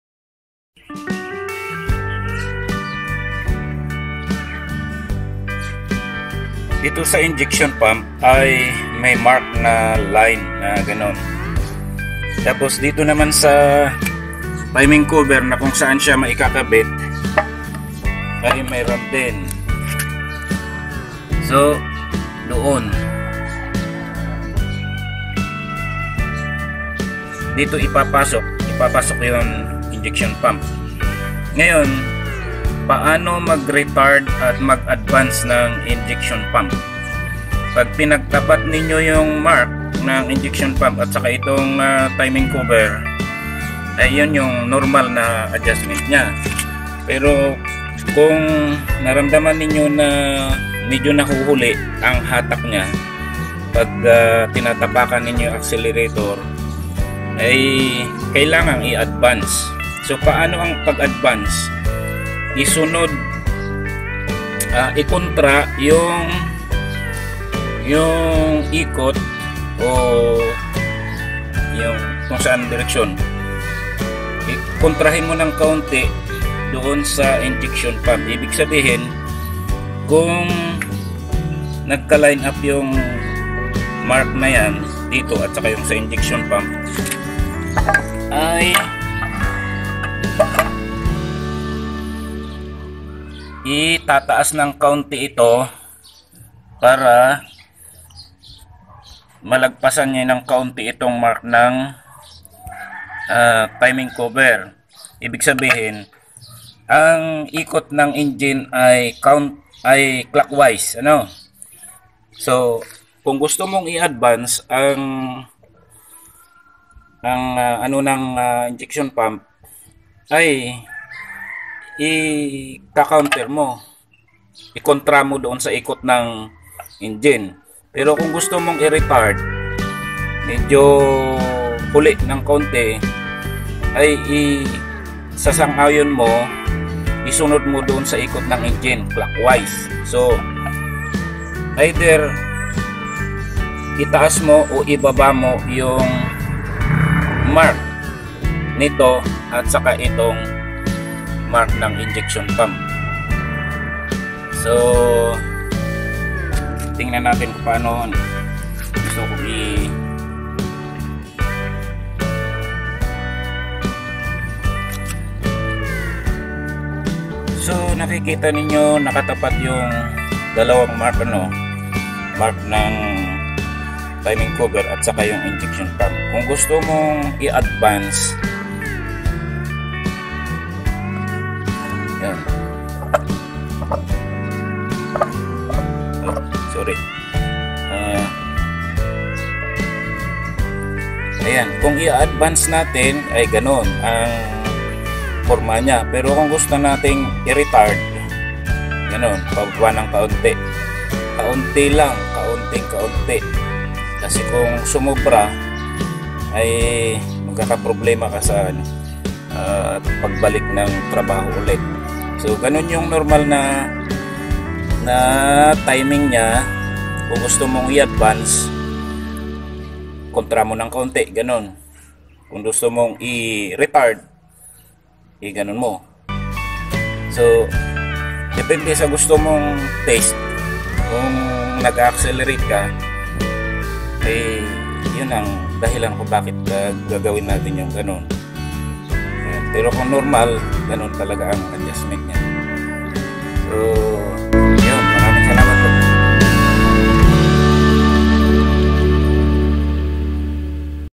Toyota L Injection Pump Advance or Retard Mode
The advance or retard mode applies in the engine ignition to bring power to the whole engine.
It depends on what you like and what is best for your engine ignition to bring best combustion of fuel and results to efficiency of power as you accelerate.

If you want a little late when you accelerate then you retard it. But if you want the engine to work a little advance when you accelerate then you set it on the advance mode.